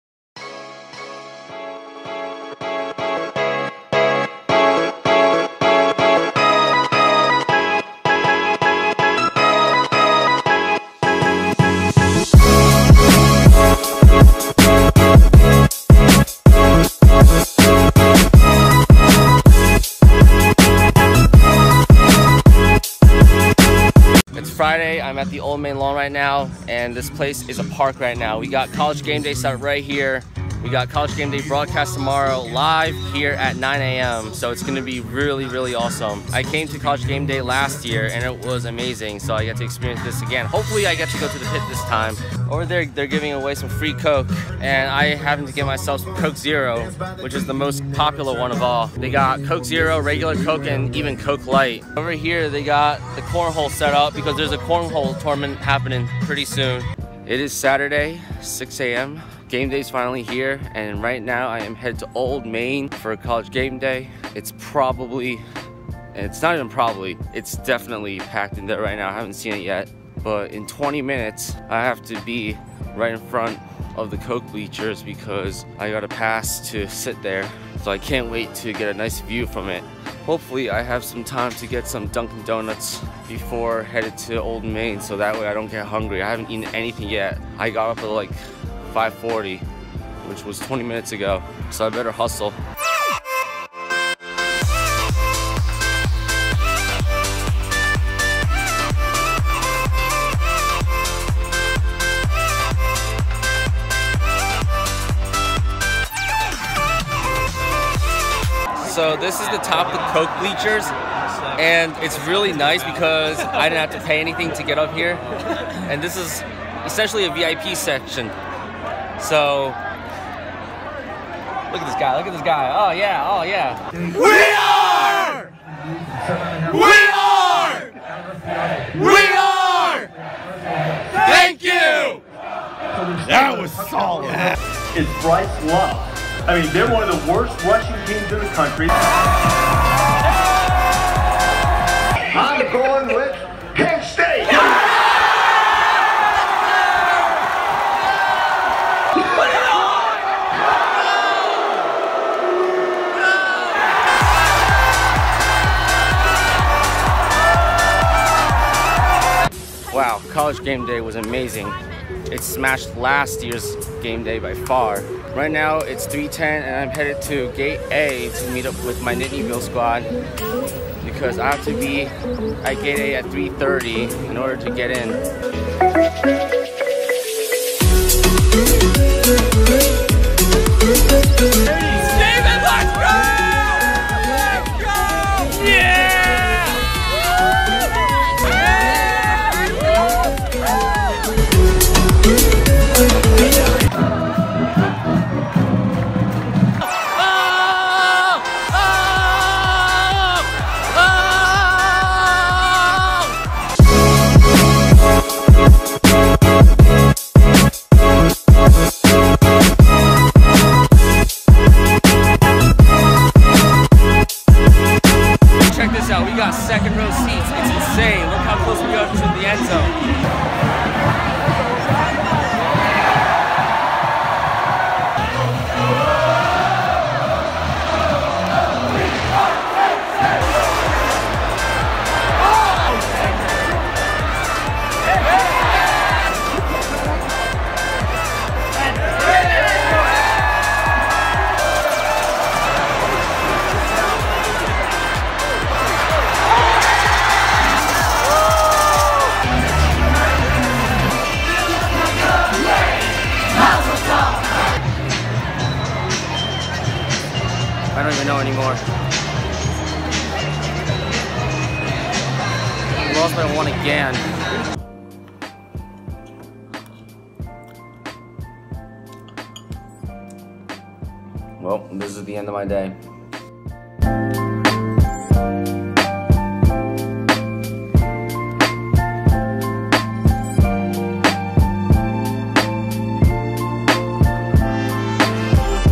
I MET JAMES FRANKLIN!
What's being a die-hard Penn State football fan without meeting Coach Franklin & football players, camping out for front row seats at the White Out, and having the best vantage point on College GameDay?

Filmed with iPhone 6 Plus